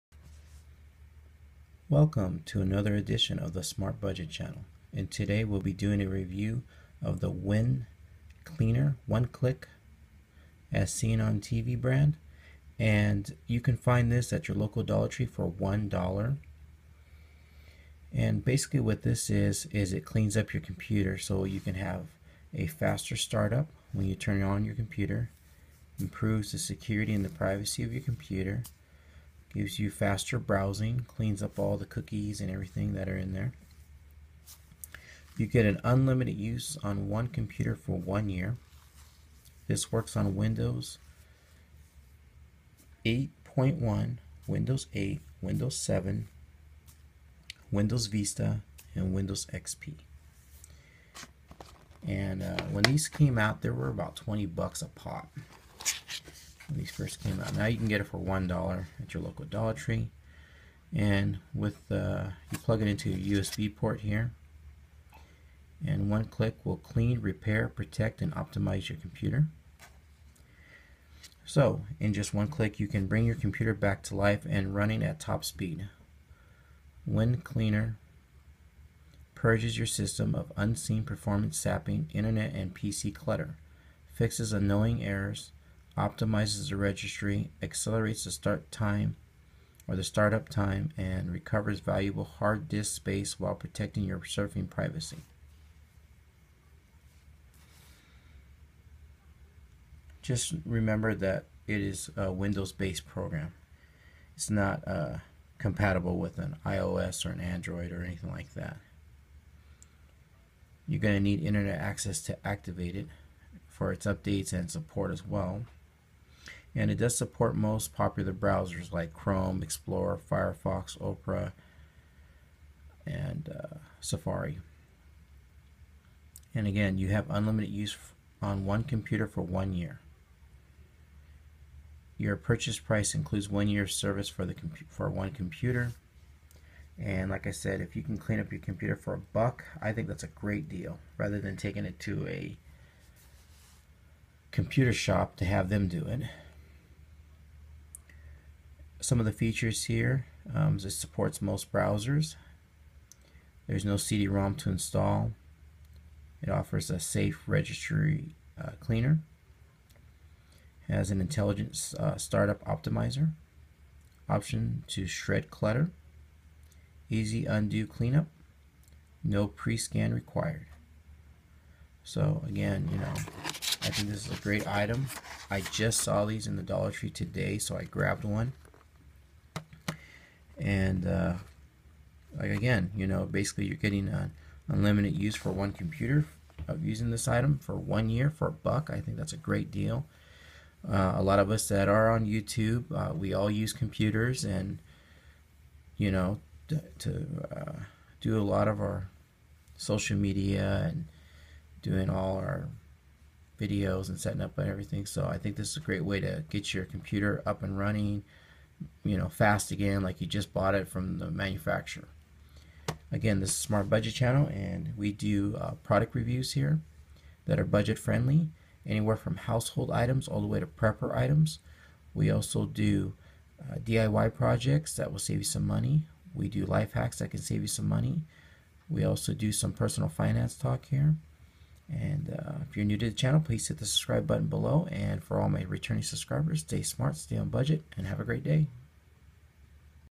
Win Cleaner - As Seen On TV review
This is my review on the Win Cleaner - As Seen On TV. This item can be picked up at your local Dollar Tree Store. You'll be able to clean up your computer and get it running fast and smooth again.